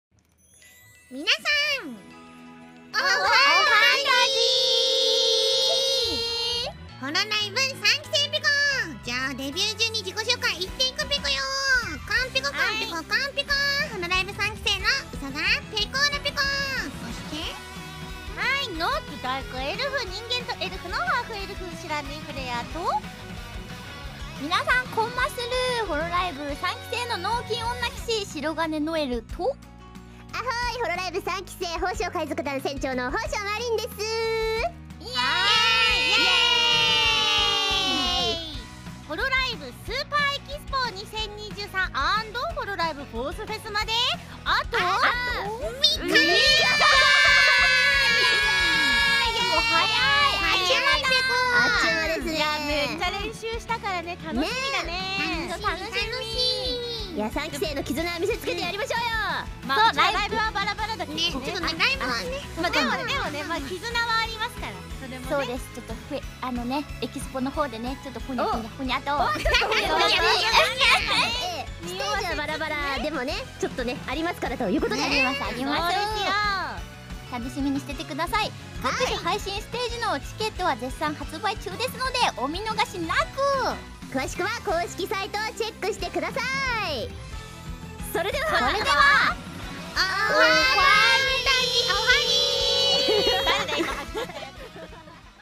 3期生來宣傳囉~距離Hololive 4th Fes.只剩下3天啦!!! 超級期待!! 【不知火芙蕾雅/白銀諾艾爾/寶鐘瑪琳/兔田佩克拉】
※本片未開營利

謝謝 :D